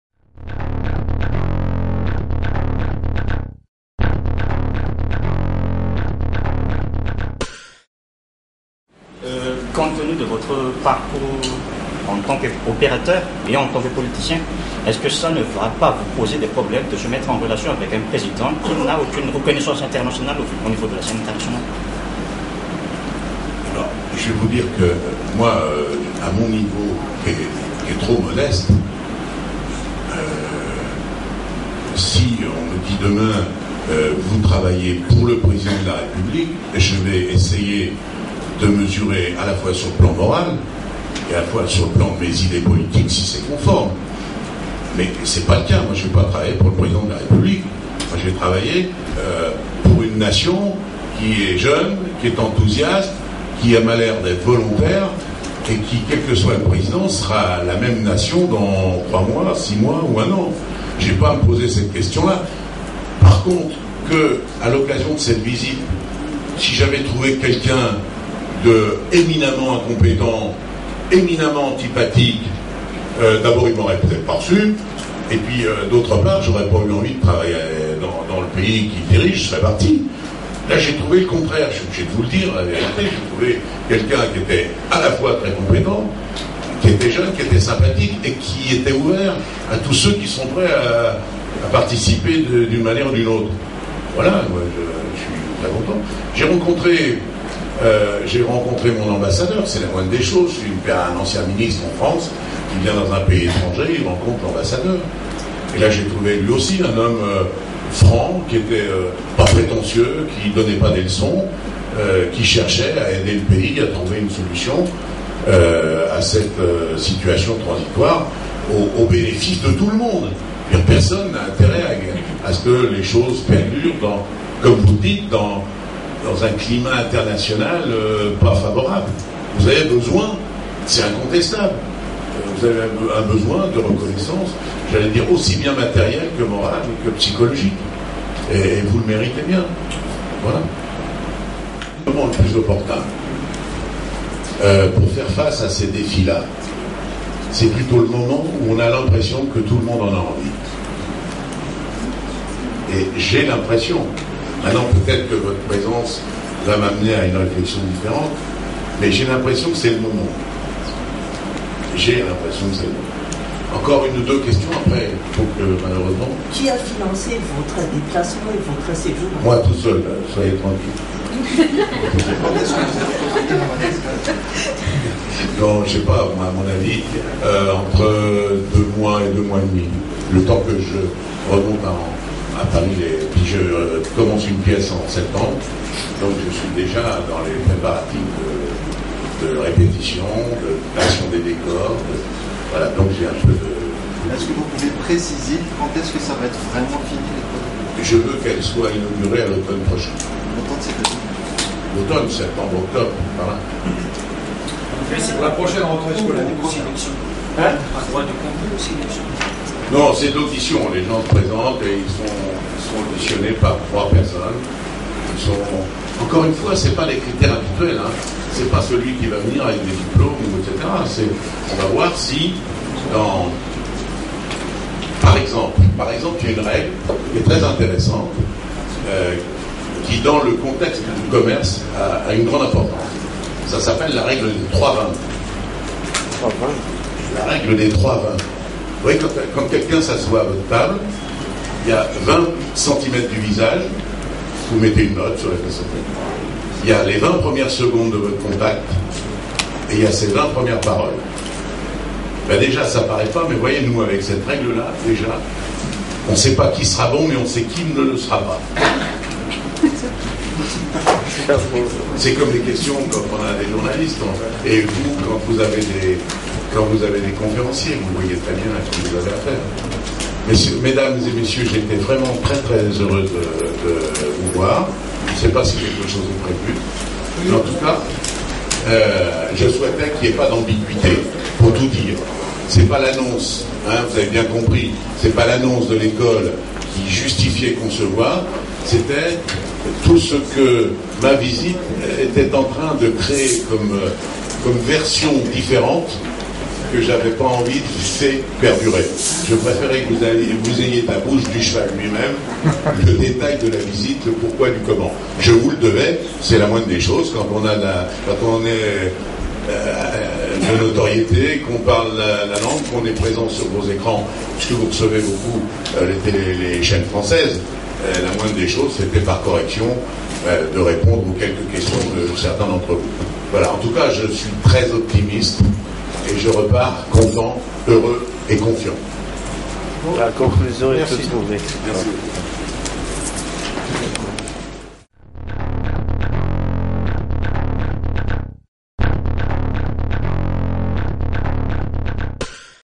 Bernard Tapie à Madagascar 8 Serein
19 janvier 2011. Point de presse à l'hôtel Colbert Antananarivo, au terme de la visite éclair de Bernard Tapie à Madagascar